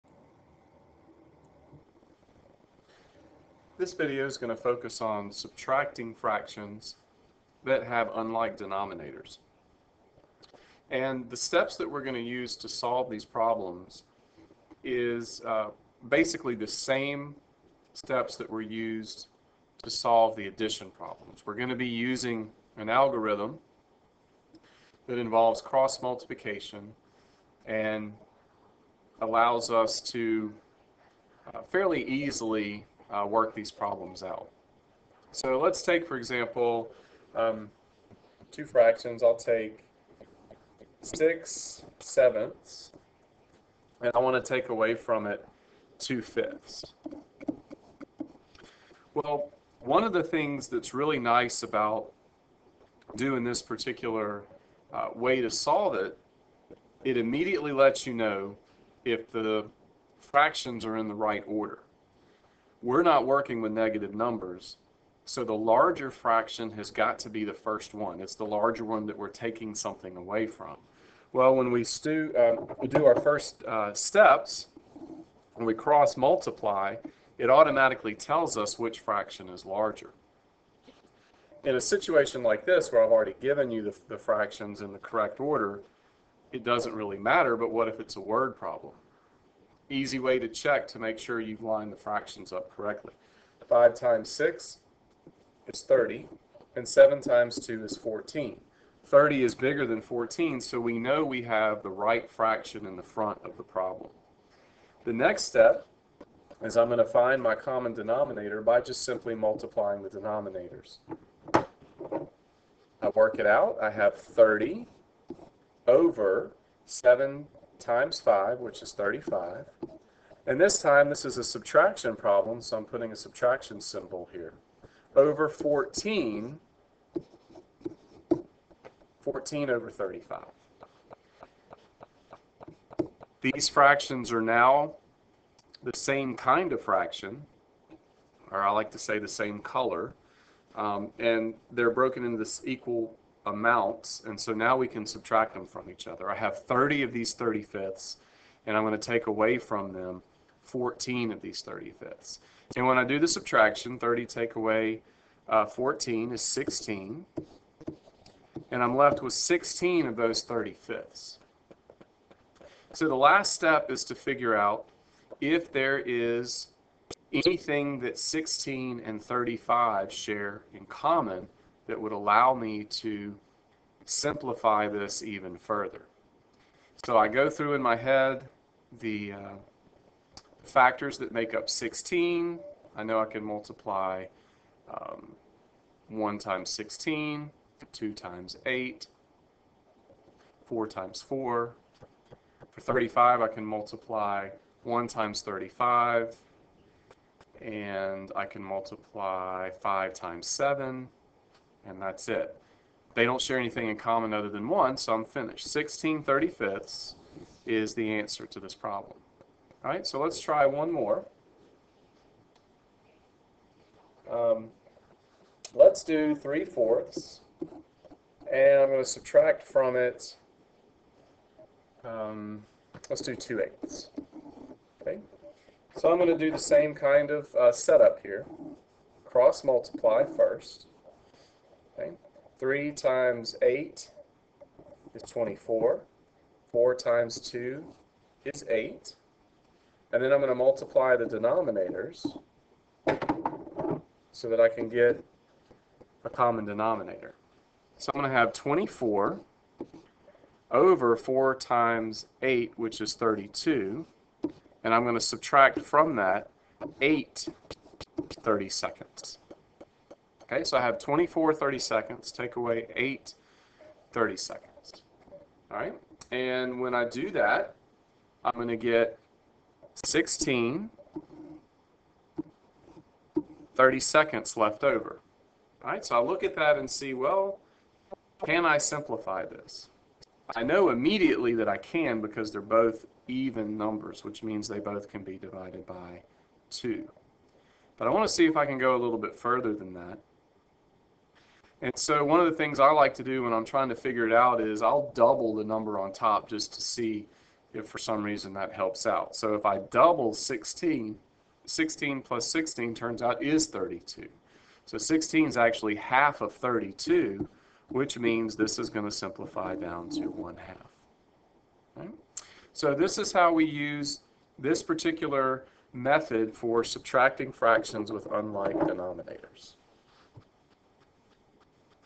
Subtracting Fractions Algorithm
This video demonstrates using the cross multiplication method for subtracting fractions.